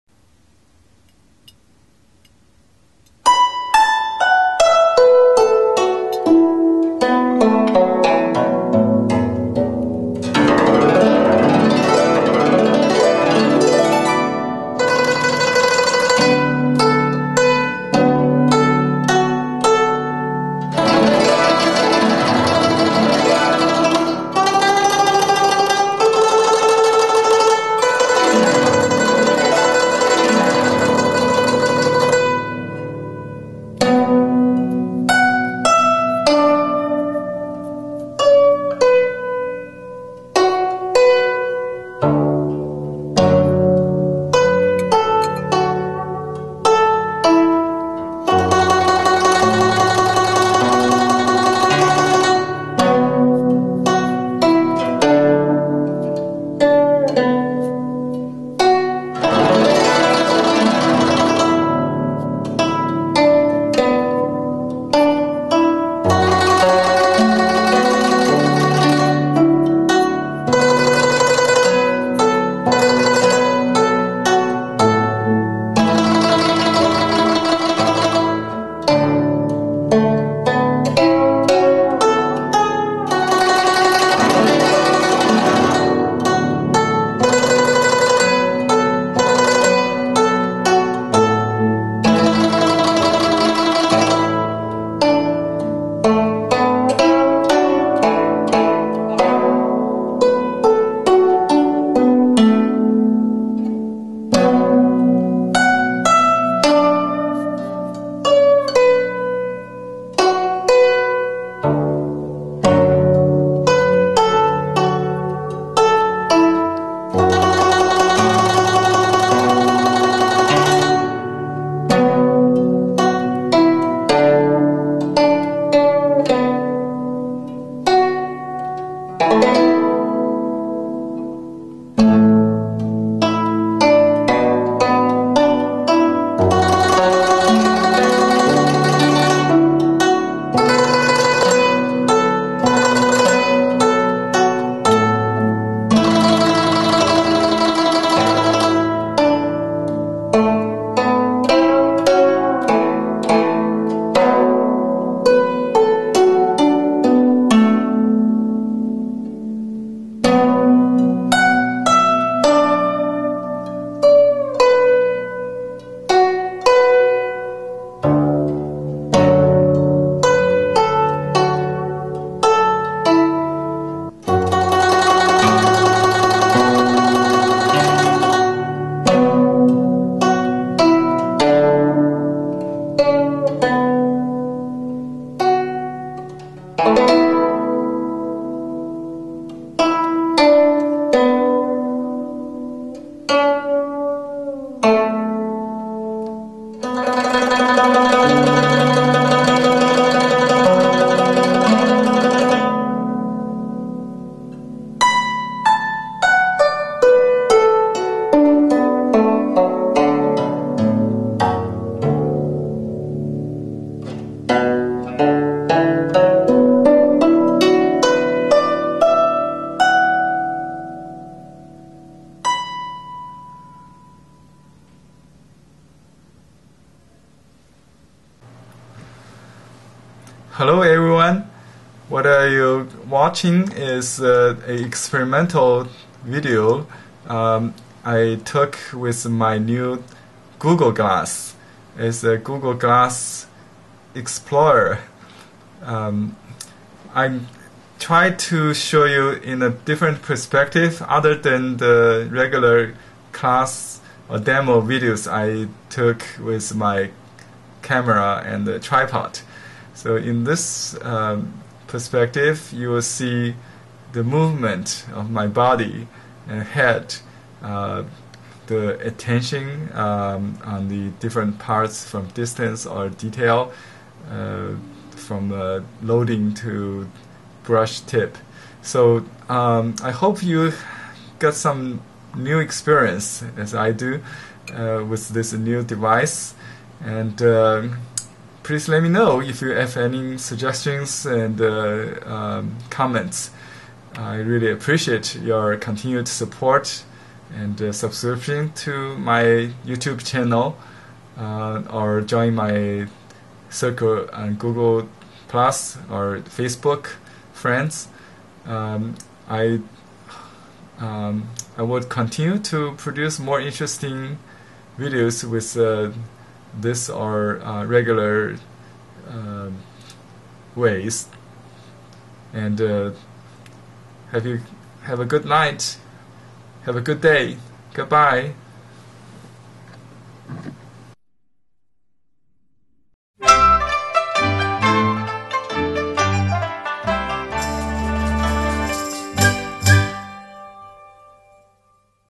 My First Google Glass Painting Video(1/2)
Materials List
BHA Mulberry Paper #2
 Combination Brush
Ox-ear Hair Brush
Sumi Ink

Background Music played by Victoria Li